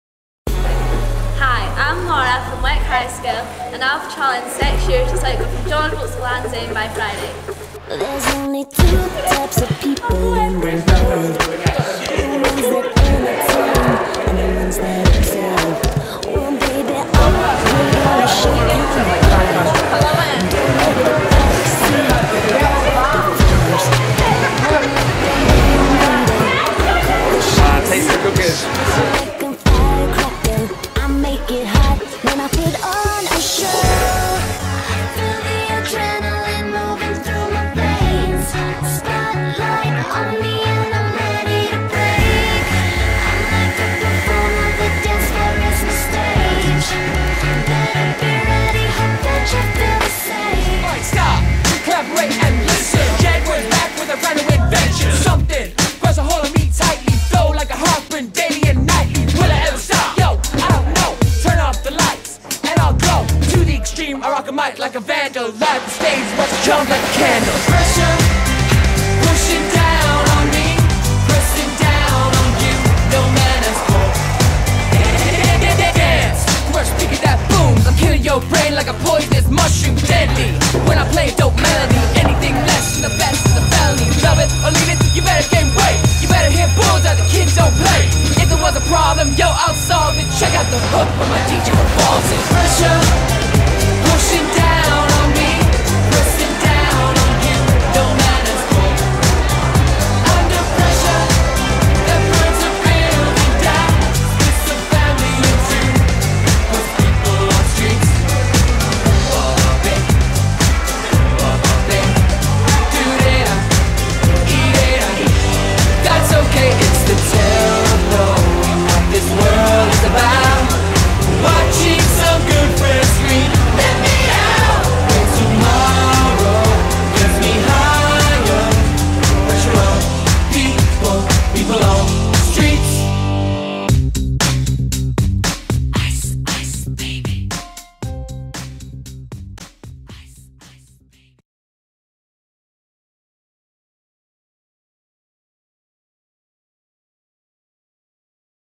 Wick High School Children in Need 2010 - Bikes & Bakes
One of four short featurettes from the Wick High School Children in Need concert/week DVD available on request—email . Should be on BBC website too...